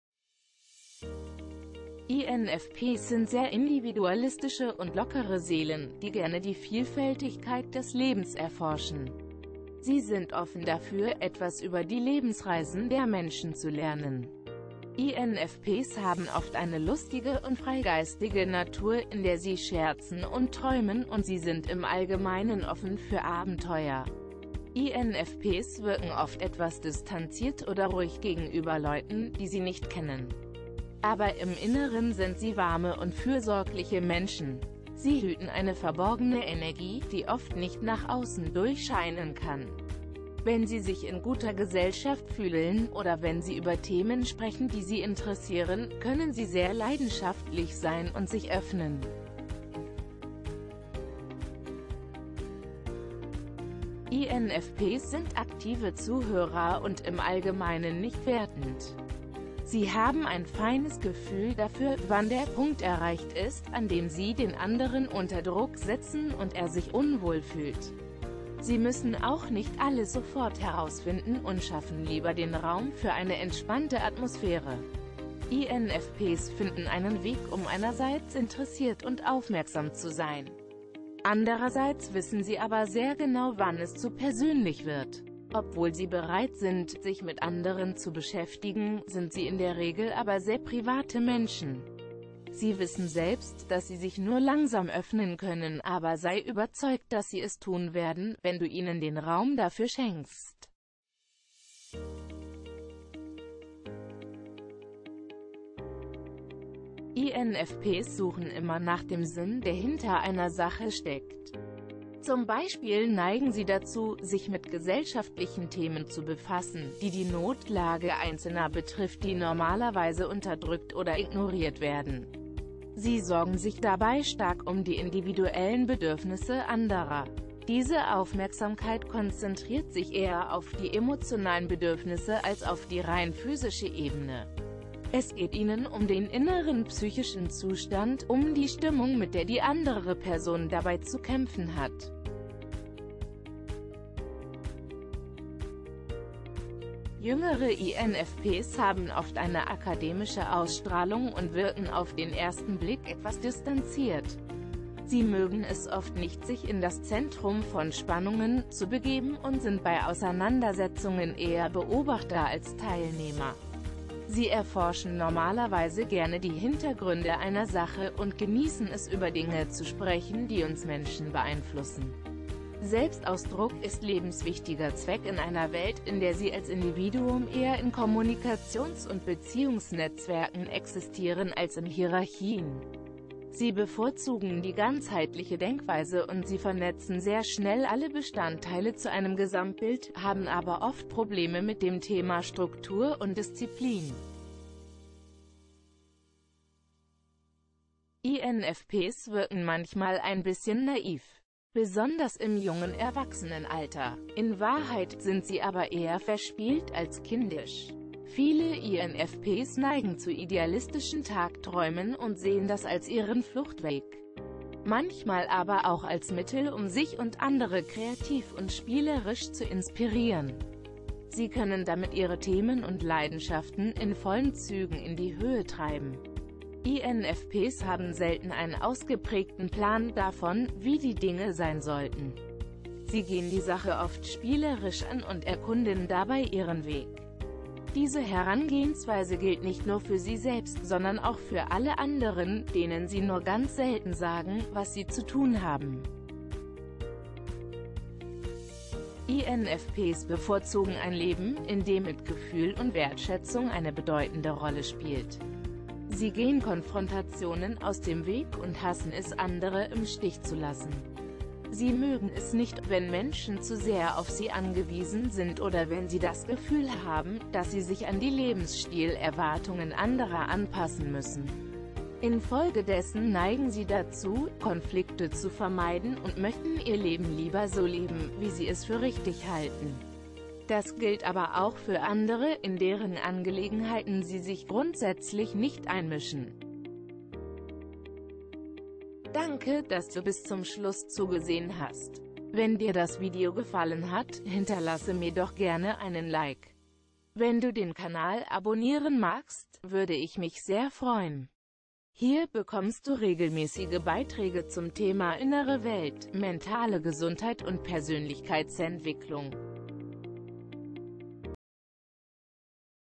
INFPs erklärt in 5 Minuten
INFPs ist eine Persönlichkeit des Meyers-Briggs-Typenindikator. Hier erfährst du in 5 Minuten die wichtigsten Merkmale dieses Typus.